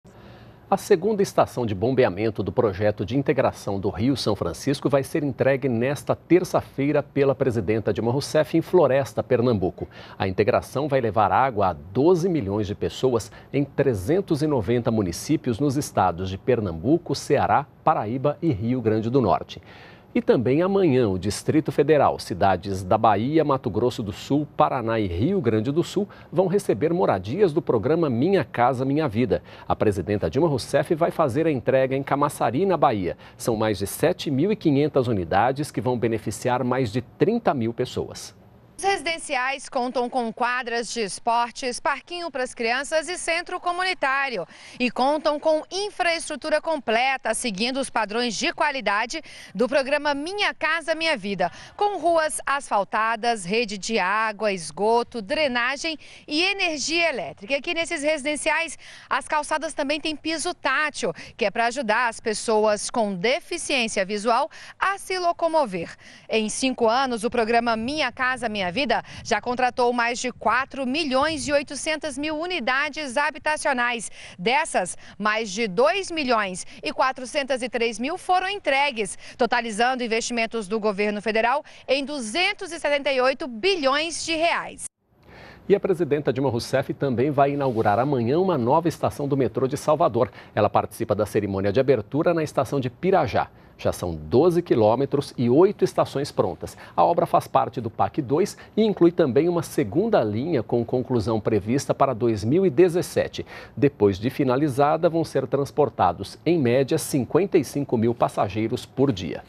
Presidenta Dilma faz entrega do Minha Casa Minha Vida em Camaçari
NBR NOTÍCIAS - 21.12.15: A segunda estação de bombeamento do projeto de integração do Rio São Francisco vai ser entregue, nesta terça-feira (22), pela presidenta Dilma Rousseff em Floresta, Pernambuco. A integração vai levar água a 12 milhões de pessoas em 390 municípios nos Estados de Pernambuco, Ceará, Paraíba e Rio Grande do Norte. E também, o Distrito Federal, cidades da Bahia, Mato Grosso do Sul, Paraná e Rio Grande do Sul vão receber moradias do Programa Minha Casa Minha Vida. A presidenta Dilma Rousseff vai fazer a entrega em Camaçari, na Bahia. São mais de 7 mil e quinhentas unidades, que vão beneficiar mais de 30 mil pessoas.